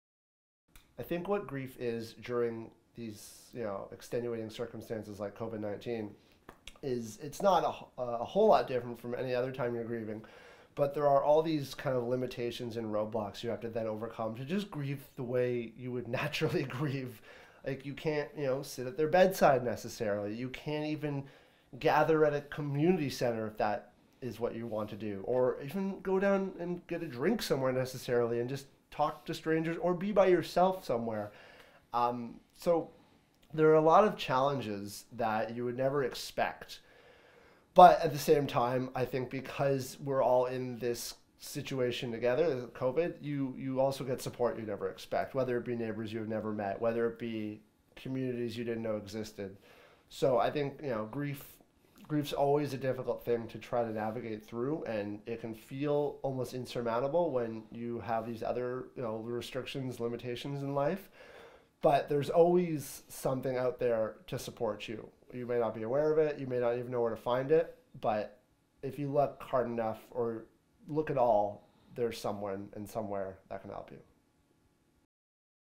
Karyn and Aidan - Describing grief during the pandemic
Karyn and Aidan talk about how their grief has changed. They also share about the challenges they have faced grieving during the pandemic.